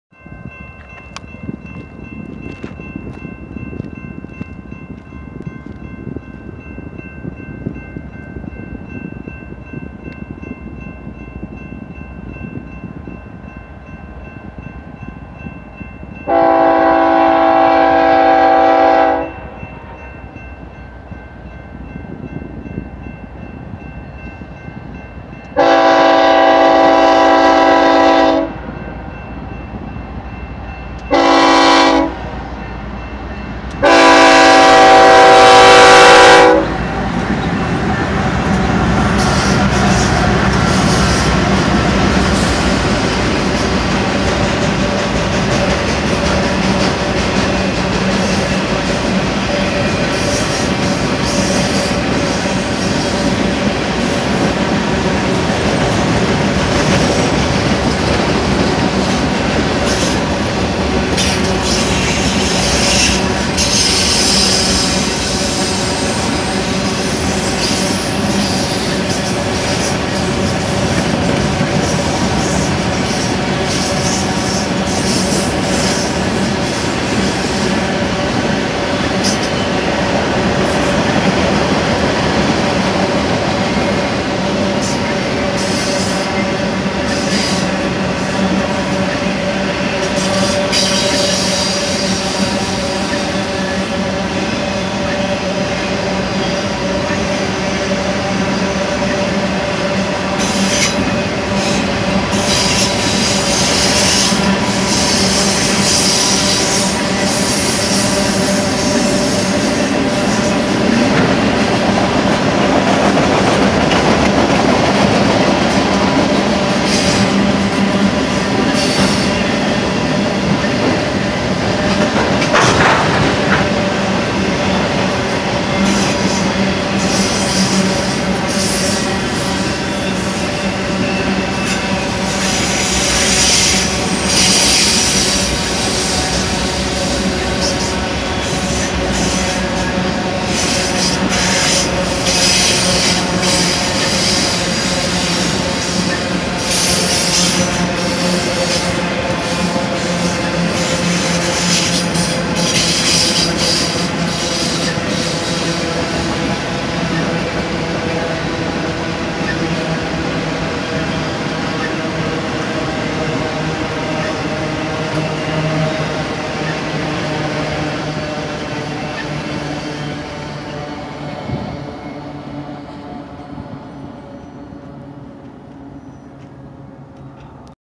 EB BNSF Autoracks Train In Hodge Ca!
A eastbound BNSF autoracks train went through here in Hodge Ca!

Here you will see in this train video is BNSF autoracks train like autorack cars

Front Engines
5948-ES44AC (BNSF) (Leading Unit)
8157-ES44C4 (BNSF) (Trailing)
1007-Dash-9C44-9W (BNSF) (Trailing)